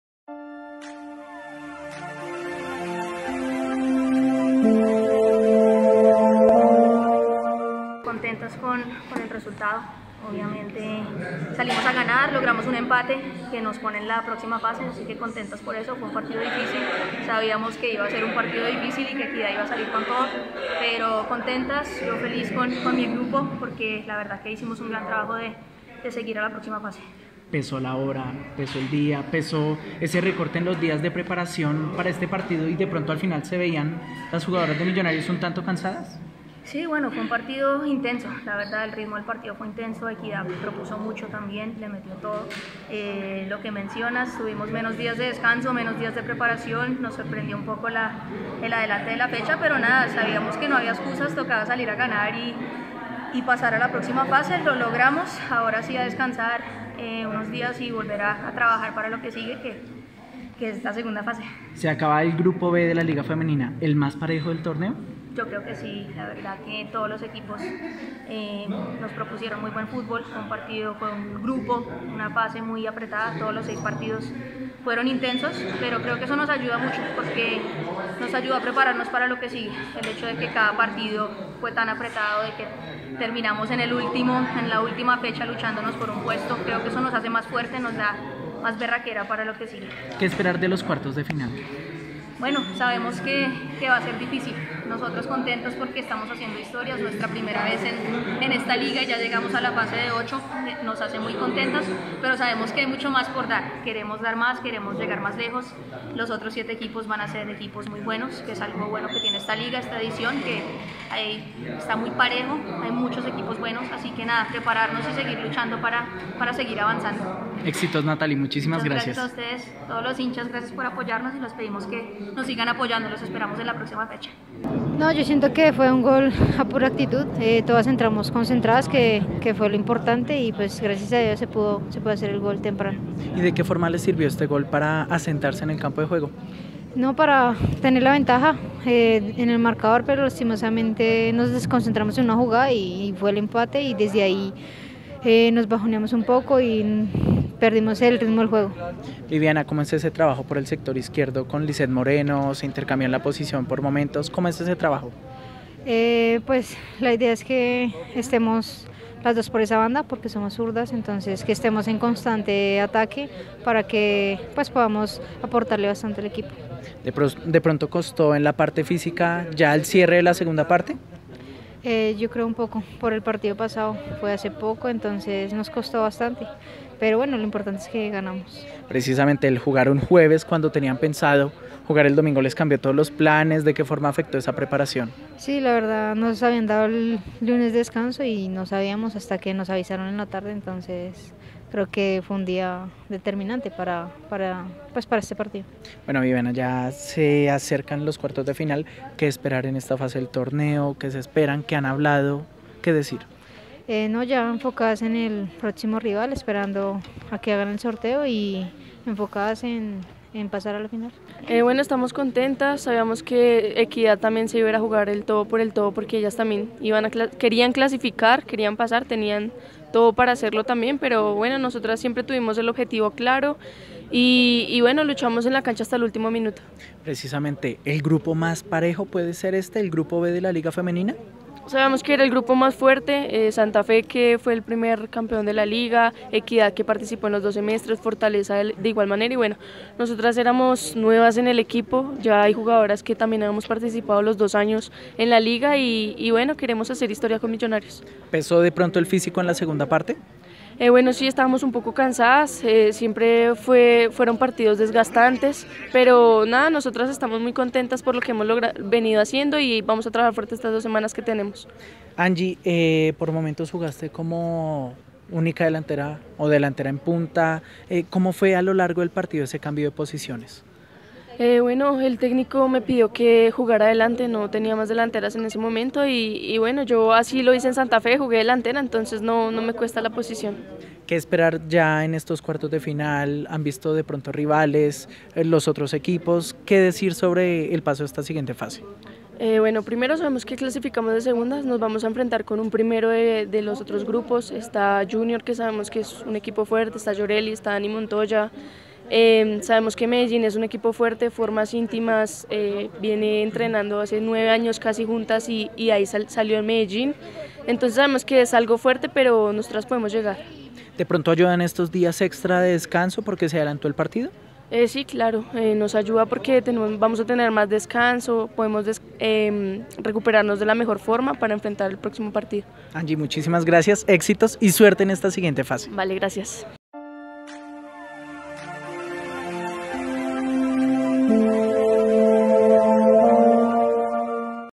Reacciones de Natalie Melo, Viviana Acosta y Angie Castañeda, tras el empate 1-1 ante Equidad
Las "Embajadoras" consiguieron el paso a los cuartos de final de la Liga Femenina.